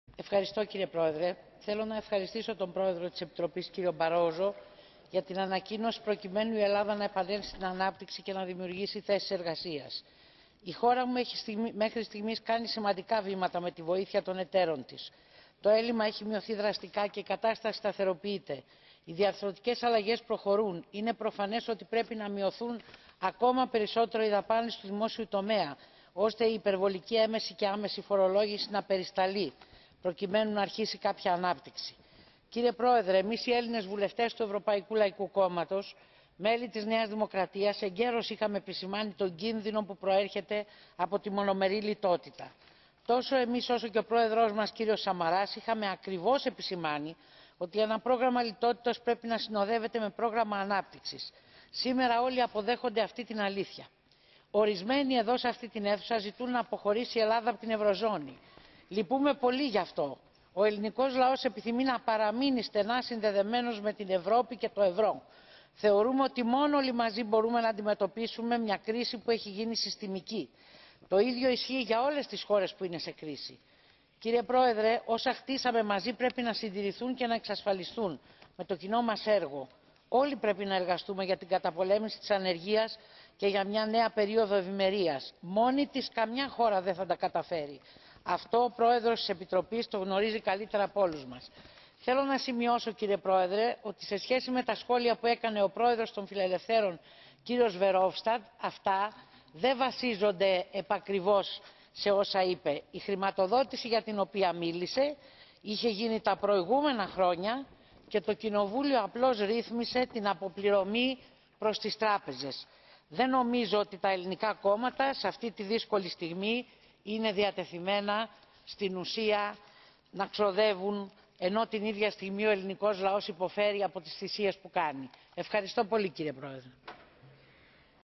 Παρέμβαση κ. Γιαννάκου για την κρίση στην ευρωζώνη
Στην Ολομέλεια του ΕΚ στο Στρασβούργο μίλησε η επικεφαλής των Ευρωβουλευτών της ΝΔ κα. Μαριέττα Γιαννάκου κατά την παρουσίαση των προτάσεων της Ευρωπαϊκής Επιτροπής για την κατάσταση στην Ελλάδα, τις προοπτικές επιστροφής στην ανάπτυξη και τη δημιουργία θέσεων απασχόλησης,  παρουσία του Προέδρου της Ευρωπαϊκής Επιτροπής κ. Μπαρόζο και του Επιτρόπου για τις Νομισματικές Υποθέσεις κ. Ρεν.

Η κα. Γιαννάκου, αφού ευχαρίστησε τον Πρόεδρο Μπαρόζο για την ανακοίνωση της Επιτροπής προκειμένου η Ελλάδα να επανέλθει στην ανάπτυξη και να δημιουργήσει θέσεις εργασίας, υπογράμμισε πως πράγματι η Ελλάδα έχει μέχρι στιγμής κάνει σημαντικά βήματα με τη βοήθεια των εταίρων της, με αποτέλεσμα το έλλειμμα  να έχει μειωθεί δραστικά και η κατάσταση να σταθεροποιείται. Όπως χαρακτηριστικά τόνισε η επικεφαλής των Ευρωβουλευτών της ΝΔ:" Οι διαρθρωτικές αλλαγές προχωρούν και είναι προφανές ότι πρέπει να μειωθούν ακόμη περισσότερο οι δαπάνες του δημοσίου τομέα, ώστε η υπερβολική άμεση και έμμεση φορολόγηση να περισταλεί προκειμένου να αρχίσει κάποια ανάπτυξη. Κύριε Πρόεδρε, εμείς, οι Έλληνες βουλευτές του ΕΛΚ, εγκαίρως είχαμε τονίσει τον κίνδυνο που προέρχεται από τη μονομερή λιτότητα. Τόσο εμείς, όσο και ο Πρόεδρός μας, κ. Σαμαράς, είχαμε ακριβώς επισημάνει ότι ένα πρόγραμμα λιτότητας πρέπει να συνοδεύεται από ένα πρόγραμμα ανάπτυξης. Σήμερα όλοι αποδέχονται αυτή την αλήθεια".

Η κα. Γιαννάκου εξέφρασε τη δυσαρέσκειά της όταν ακούγονται, έστω και ελάχιστες, φωνές στο ΕΚ που ζητούν να αποχωρήσει η Ελλάδα από την Ευρωζώνη.  Η ίδια επεσήμανε για άλλη μια φορά ότι: " Ο ελληνικός λαός επιθυμεί να παραμείνει στενά συνδεδεμένος με την Ευρώπη και το ευρώ. Θεωρούμε ότι μόνον όλοι μαζί μπορούμε να αντιμετωπίσουμε μια κρίση που έχει γίνει συστημική. Το ίδιο ισχύει για όλες τις χώρες που είναι σε κρίση. Μόνη της καμία χώρα δεν θα τα καταφέρει. Αυτό ο Πρόεδρος της Ευρωπαϊκής Επιτροπής το γνωρίζει καλύτερα από όλους μας", ενώ σε σχέση με τα σχόλια του Προέδρου της Ομάδας των Φιλελευθέρων κ. Φερχόφσταντ ως προς τη ρύθμιση των χρεών των κομμάτων προς τις τράπεζες, δήλωσε :" Δεν είναι ακριβώς έτσι τα πράγματα. Η χρηματοδότηση είχε γίνει τα προηγούμενα χρόνια και το εθνικό κοινοβούλιο απλώς ρύθμισε την αποπληρωμή προς τις τράπεζες. Δε νομίζω ότι τα ελληνικά κόμματα σε αυτή τη δύσκολη στιγμή είναι διατεθειμένα να ξοδεύουν, ενώ την ίδια στιγμή ο ελληνικός λαός υποφέρει από τις θυσίες που κάνει".